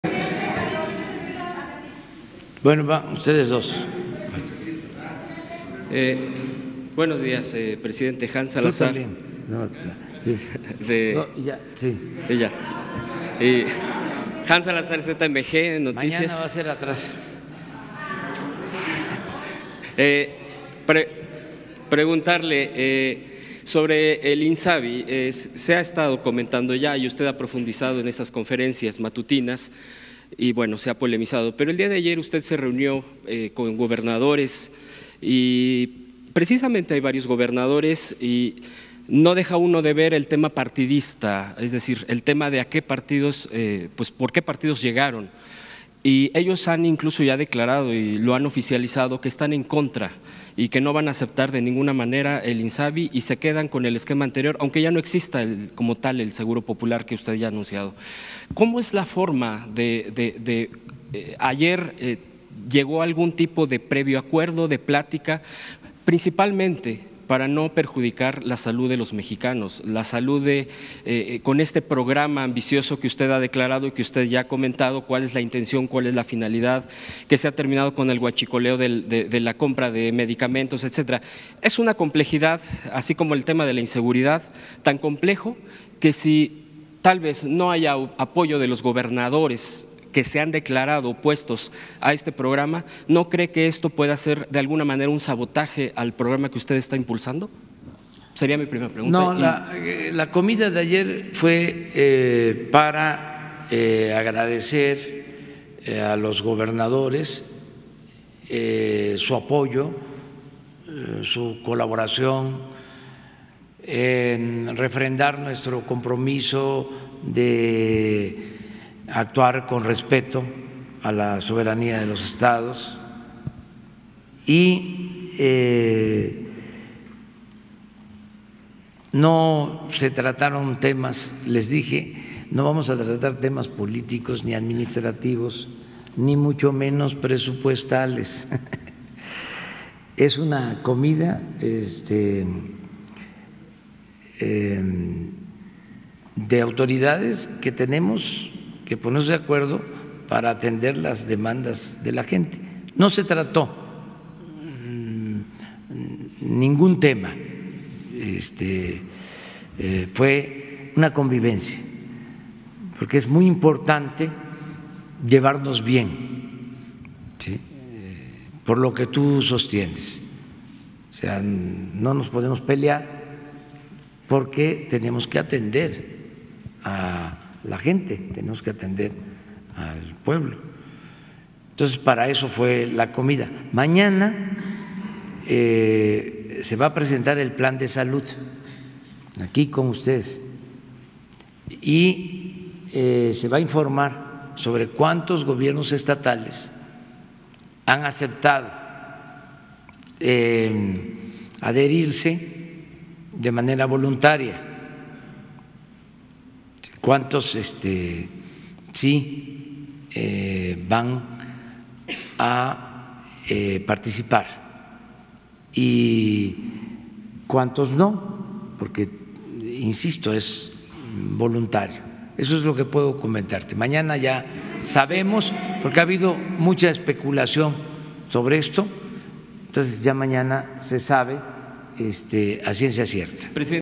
AMLO ACLARA QUE GOBERNADORES DARÁN A CONOCER MAÑANA AFILIACIÓN A INSABI
El presidente aclaró que la comida de ayer fue para agradecer a los gobernadores su apoyo y colaboración, para refrendar el compromiso de actuar con respeto a la soberanía de sus estados. López Obrador aseguró que incluso les dijo que no iban a tratar temas políticos y mucho menos presupuestales… Pero que mañana darán a conocer quiénes se afilian al Insabi